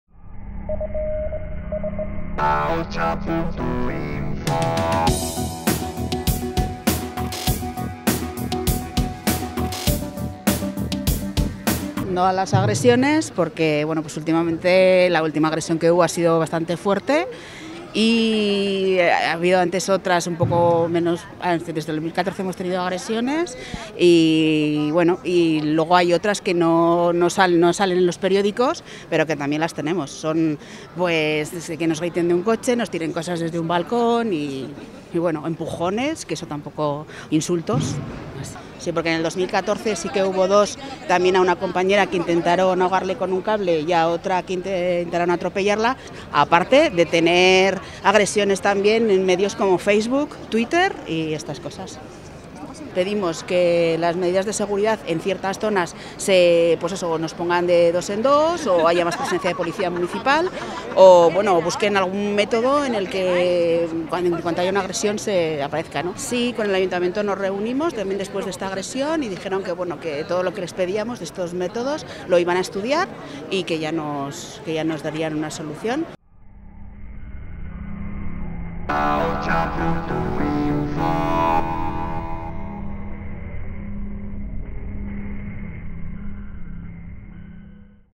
Trabajadoras de Dornier denuncian que las agresiones son continuas
Trabajadoras de Dornier, empresa que gestiona la zona azul se han concentrado denunciando la última agresión sufrida por una trabajadora.